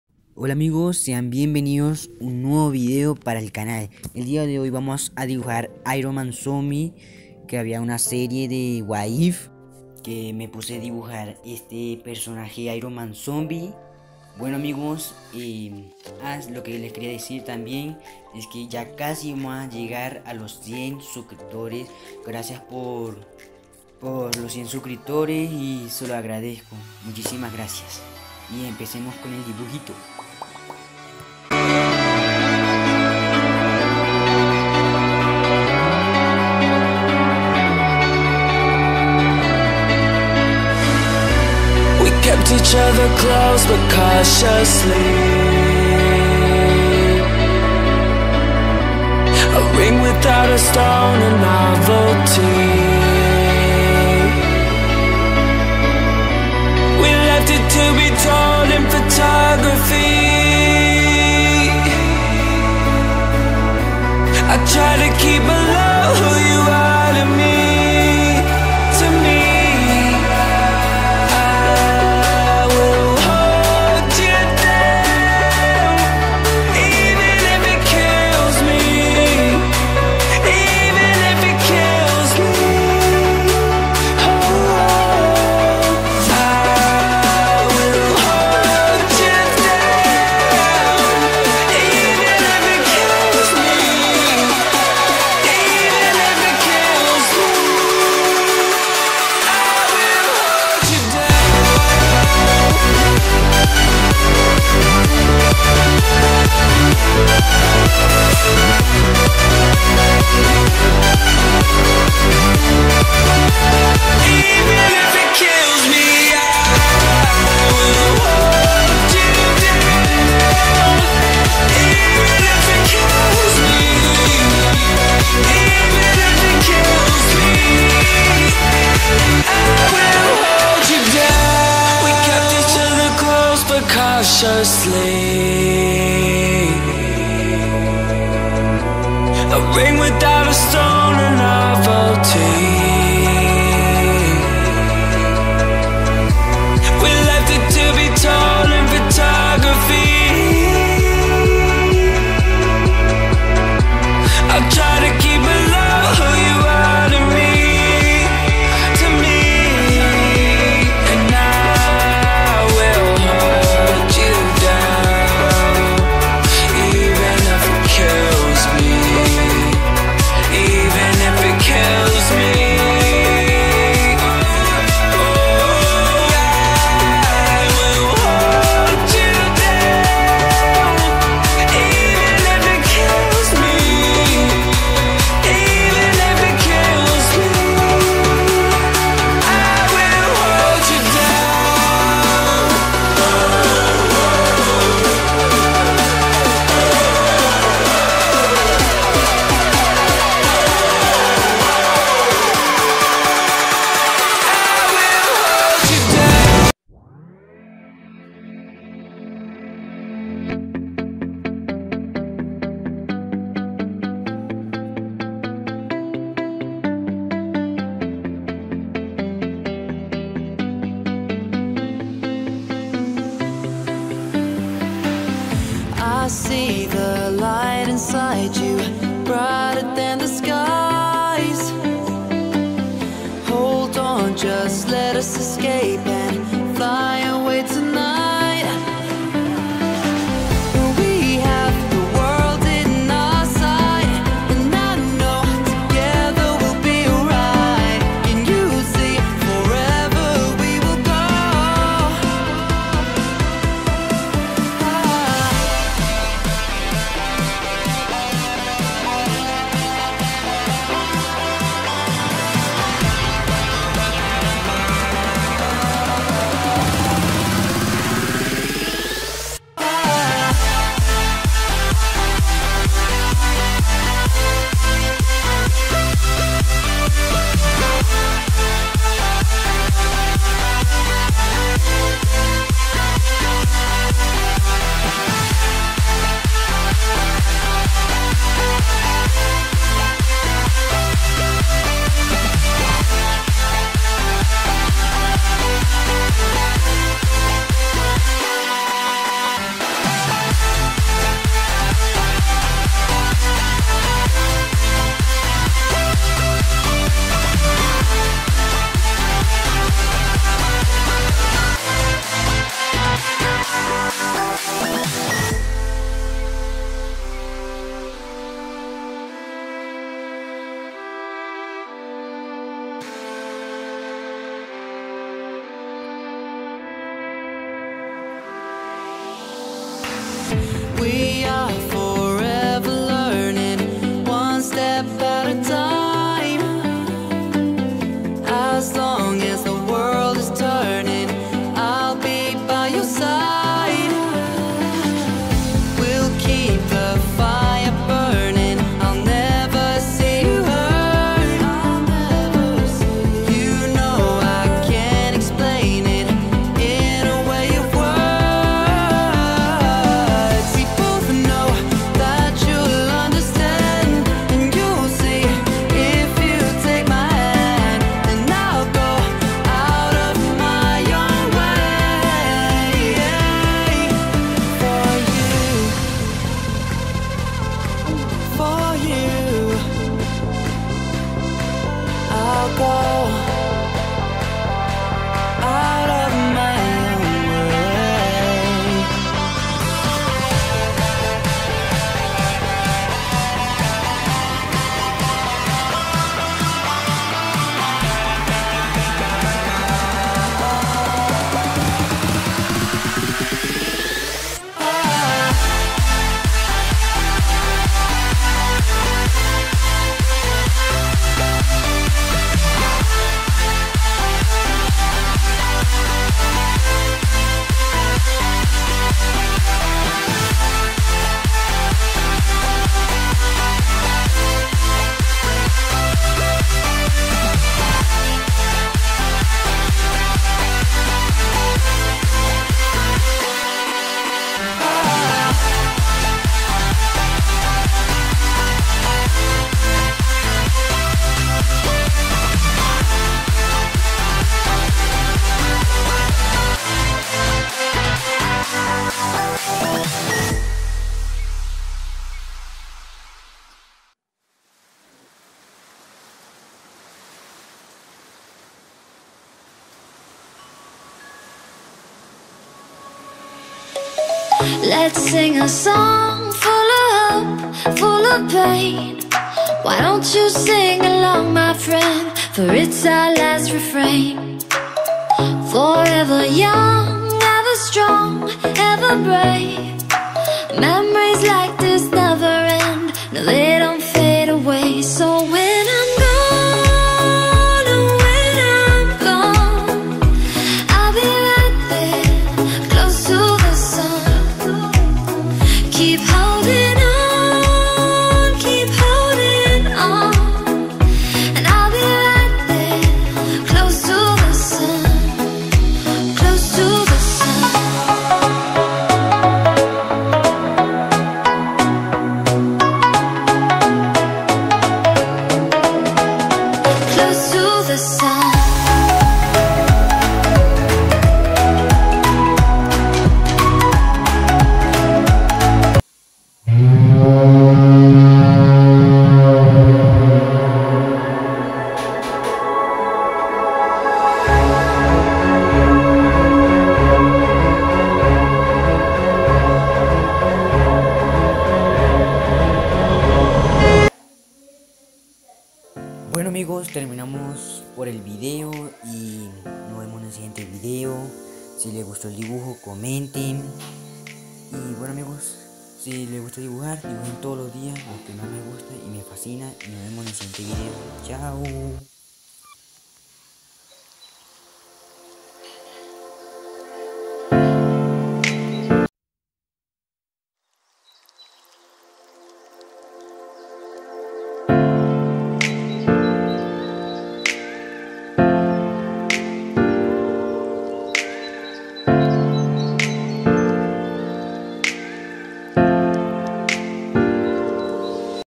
Dibujo IRONMAN ZOMBIE - Marvel - What If
En este video te enseñare a como dibujar a iron man
zombie marvel What lf

listado de materiales
hoja blanca
colores Faber Castell
estilografo desechables

estare haciendo mas video y buenos amigos aqui me despido
sigan haciendo dibujos